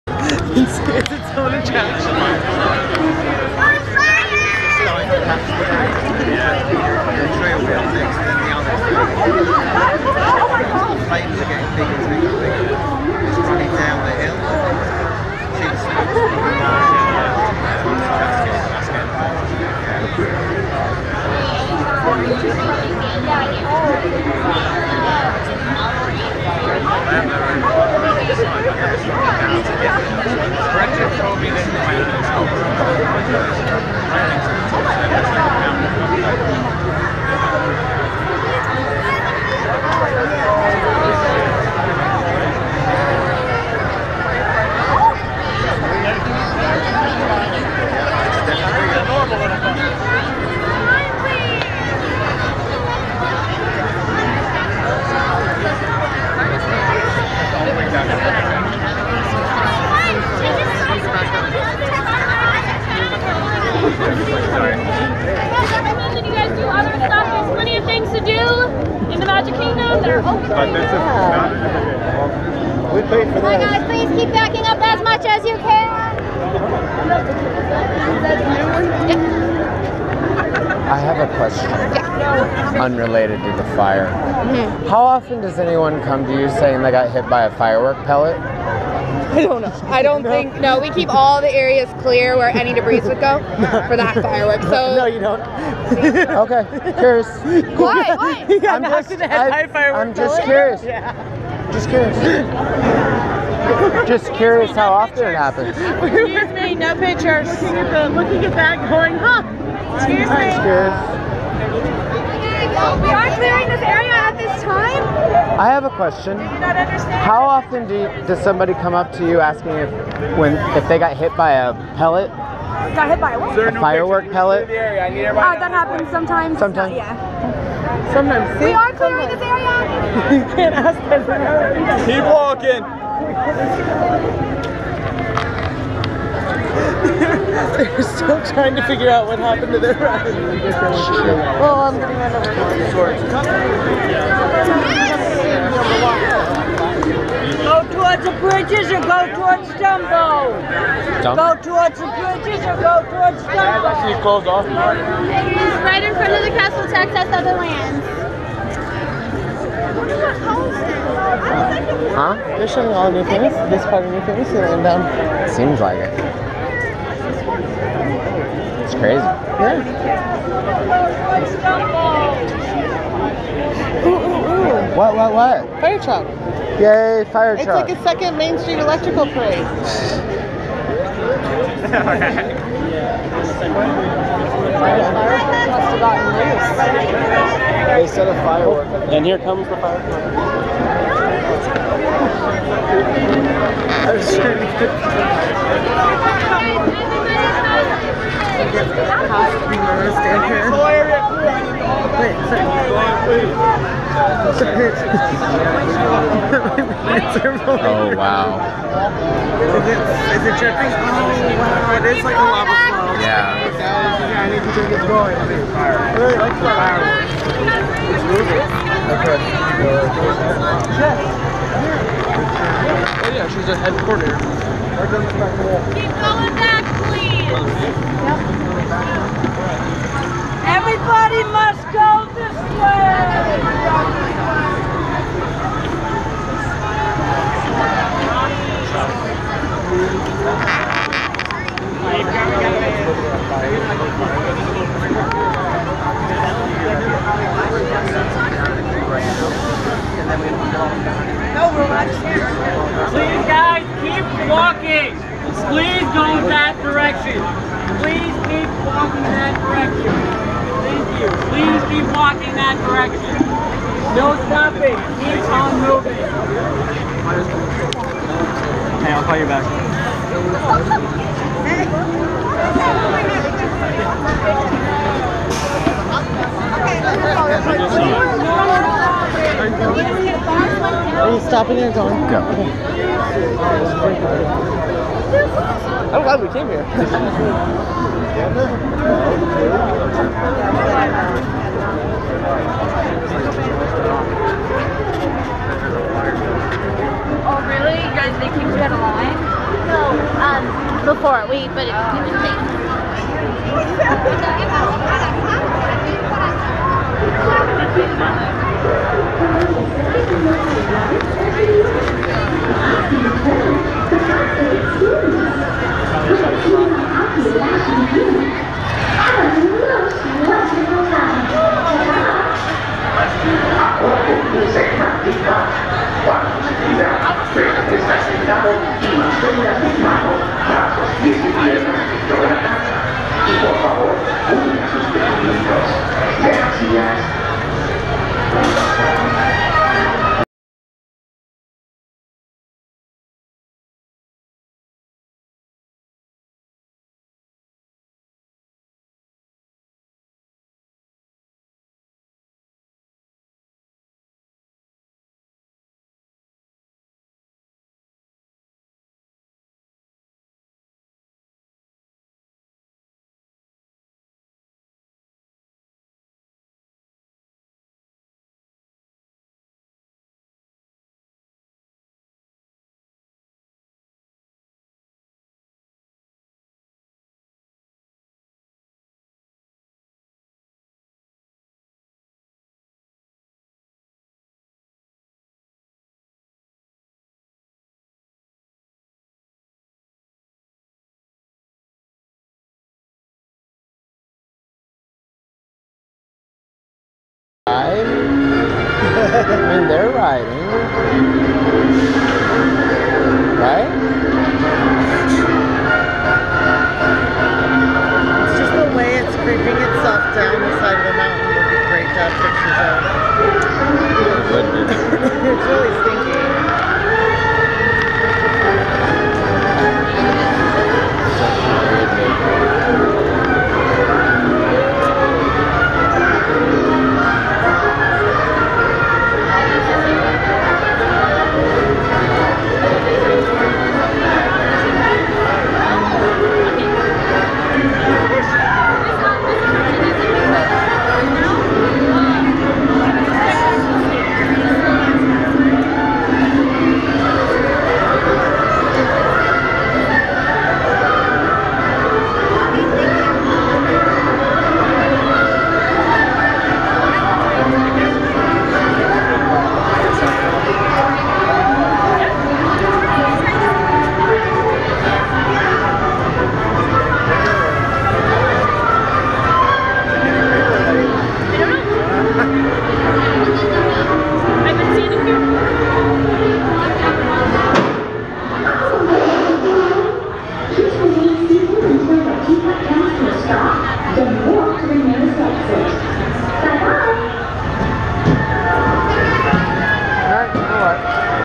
Seven Dwarfs Mine Train on Fire at the Magic Kingdom in Walt Disney World
A fire on the outside of the Seven Dwarfs Mine Train at the Magic Kingdom in Walt Disney World started after 10:30pm. According to many, firework fallout is most likely the cause of the fire that was extinguished by RCID Fire and Rescue Department. The Mine Train was reopened by midnight, no injuries were reported.